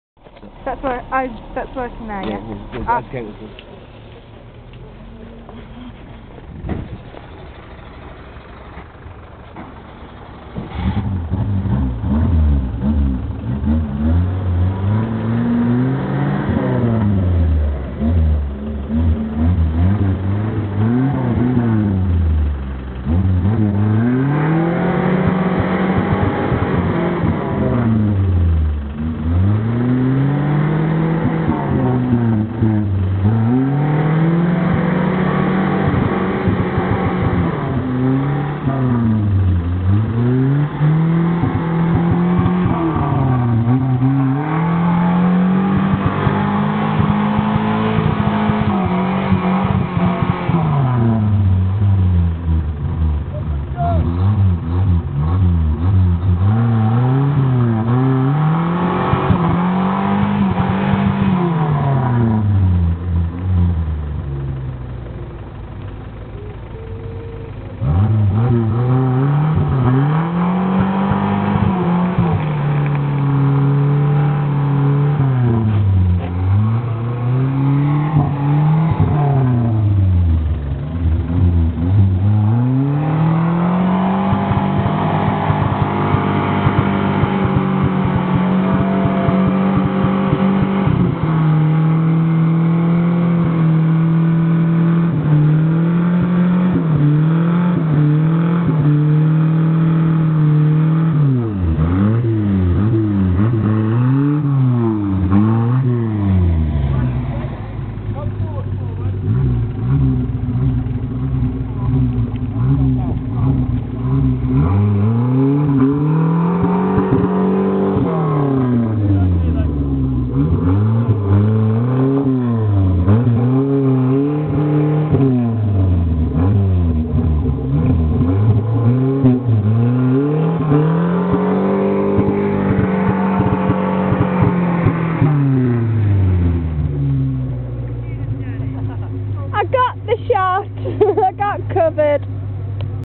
my first journey assist (cause we dont get stuck)
parently i had a brake stuck on as well, not sure how that happened!